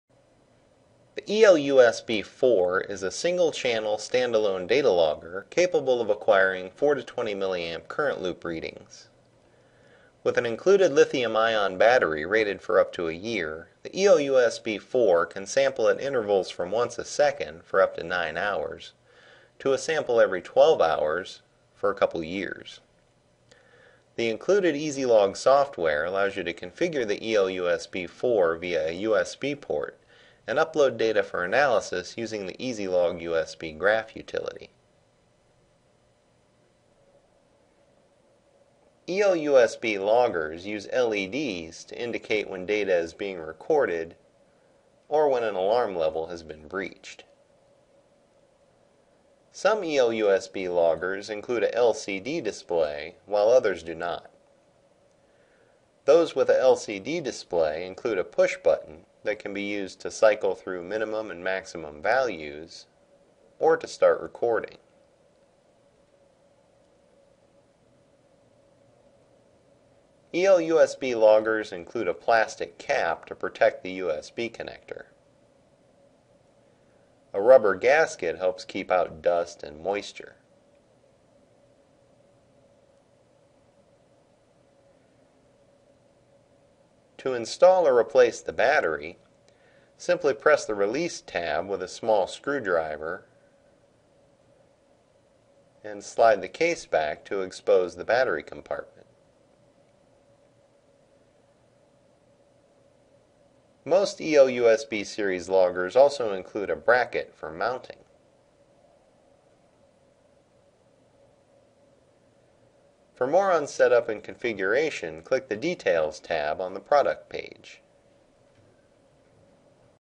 Introducing the EL-USB-4 Current Loop Data Logger
The EL-USB-4 is a stand-alone data logger capable of acquiring 4-20 mA readings.

For more information on the EL-USB-4, or to place an order, got